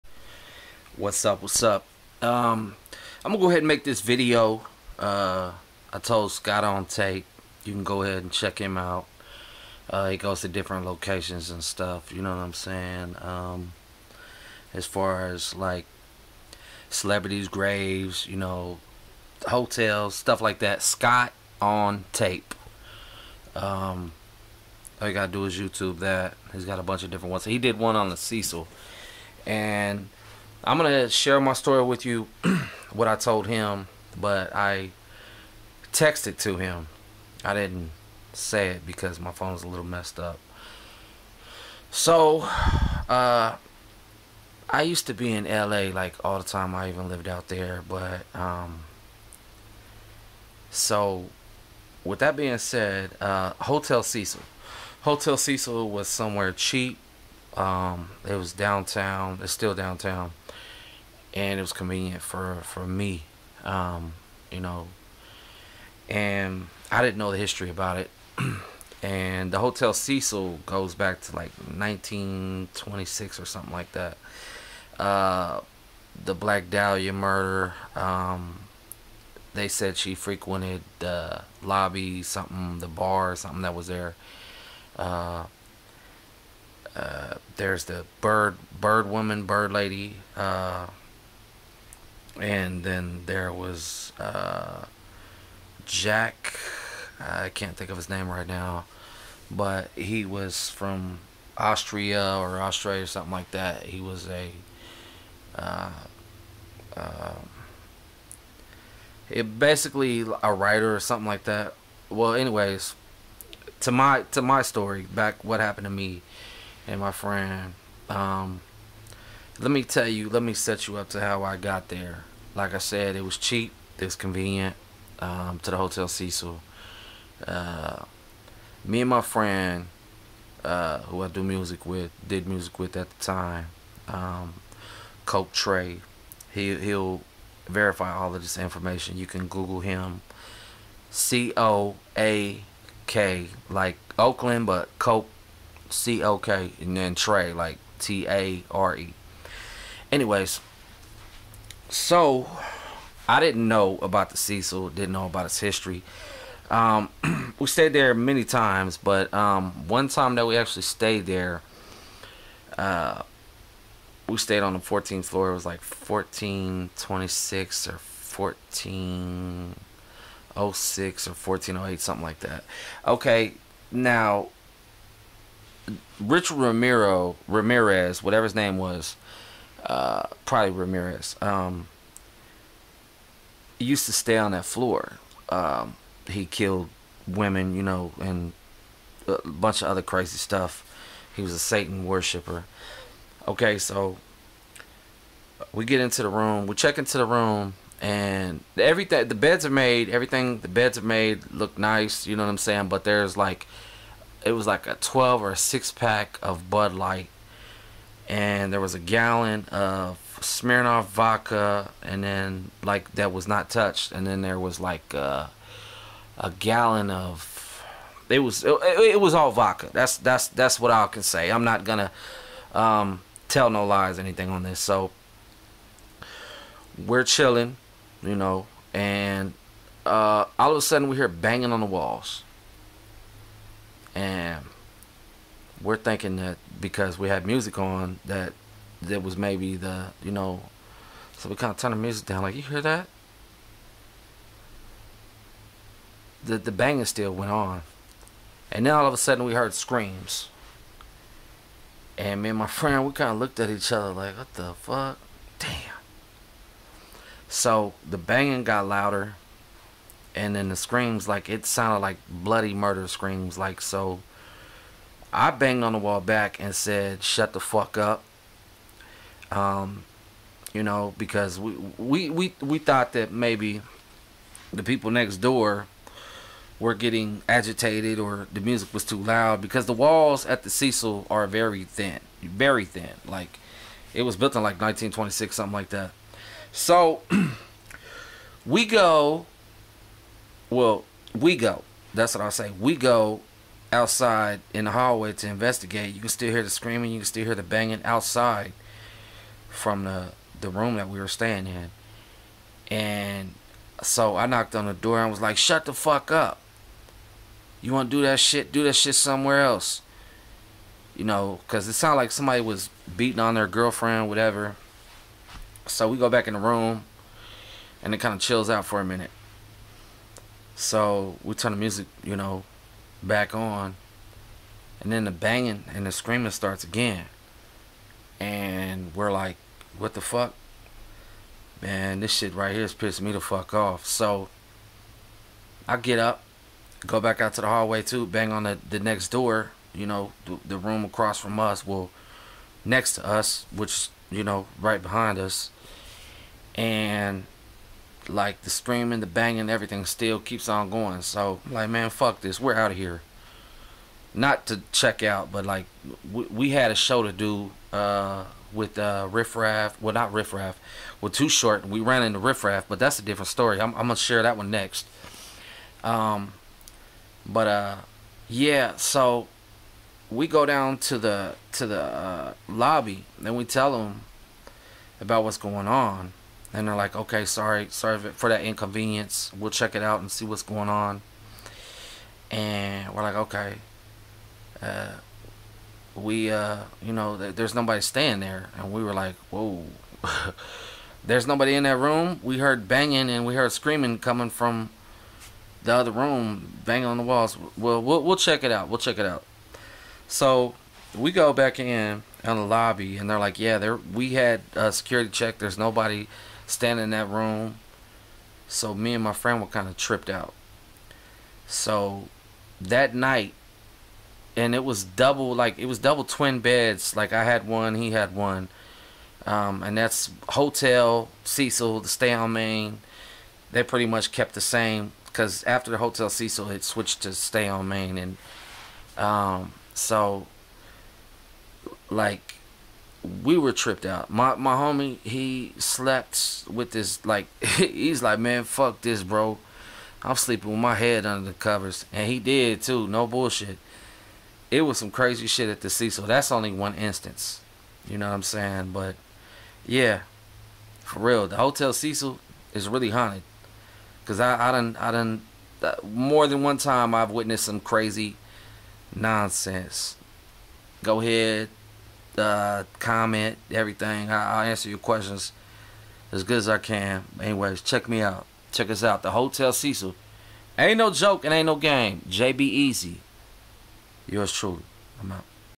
the hotel Cecil is haunted
my experience at the hotel Cecil back in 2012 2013 when I stayed there